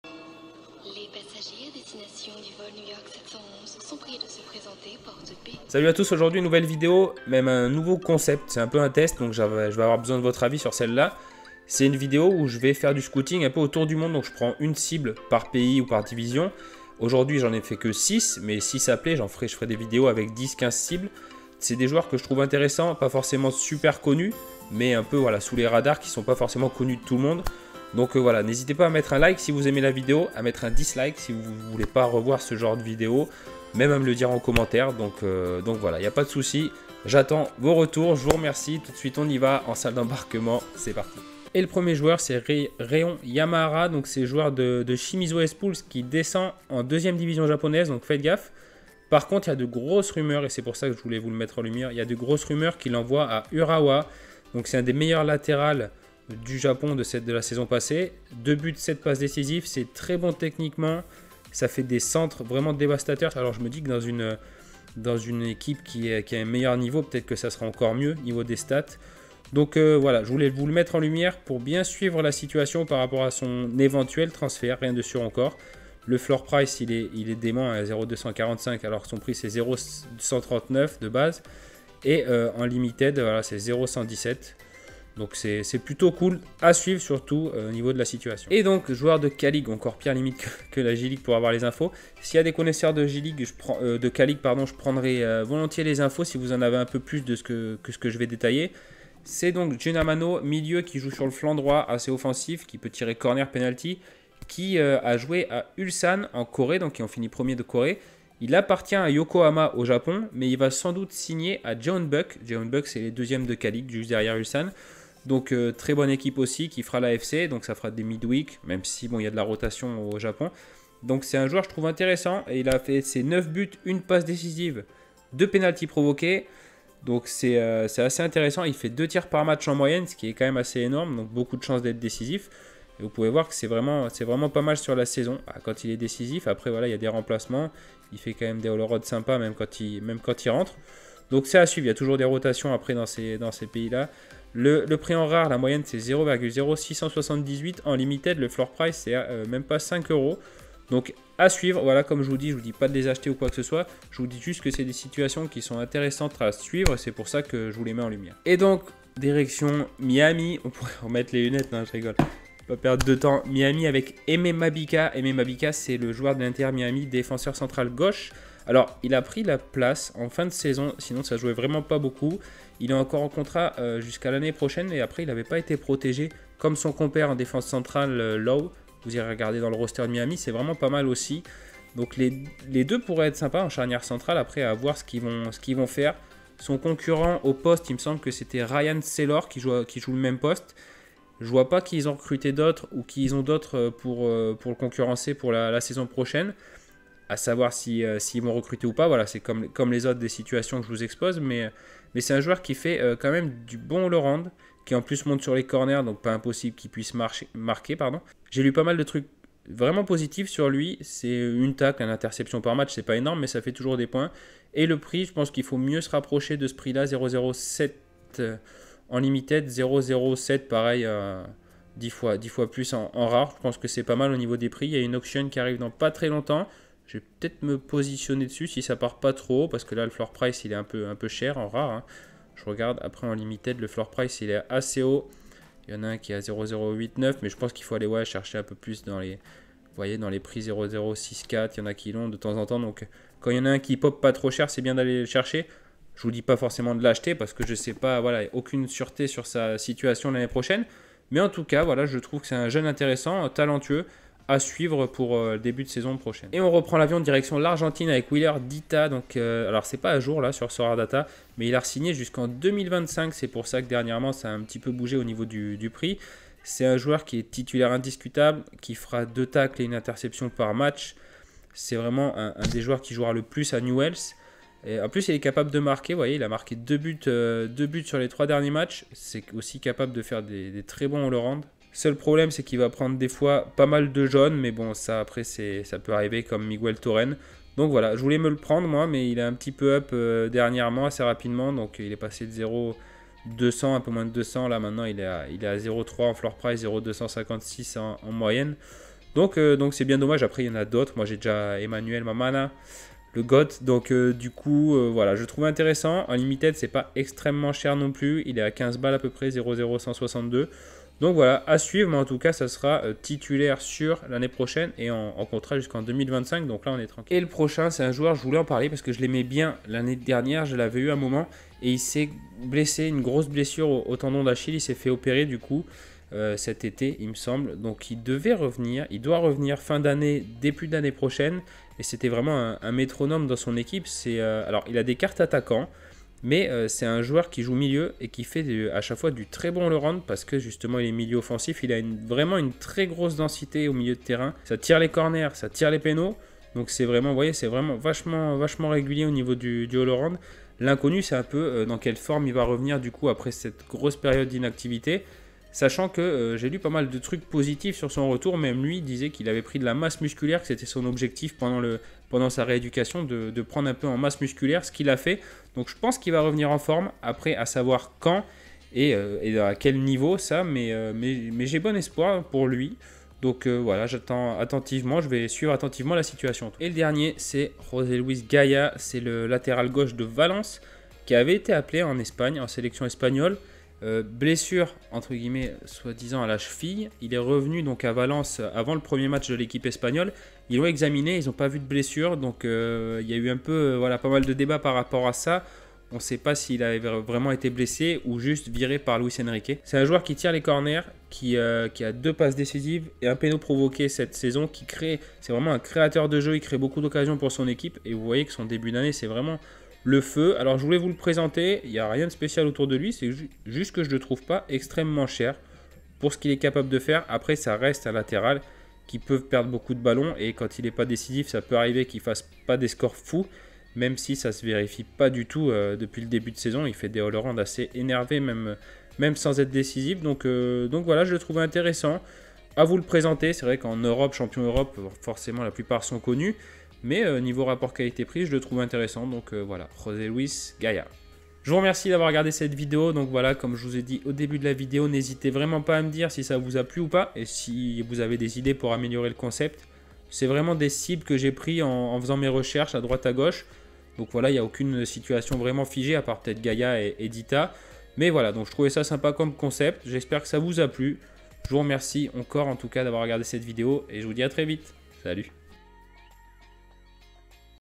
Sorare : Scouting Autour du monde 🌍
Salut la Team ! Aujourd'hui un test pour un concept qui peut être sympa, j'attends de voir si ça vous plais, un scouting autour du monde avec (aujourd'hui) 6 joueurs de 6 championnats différents, alors attachez vos ceintures on décolle !!!

Lien de parrainage (1 carte cadeau en passant par ce lien après 5 enchères + un super parrain :D)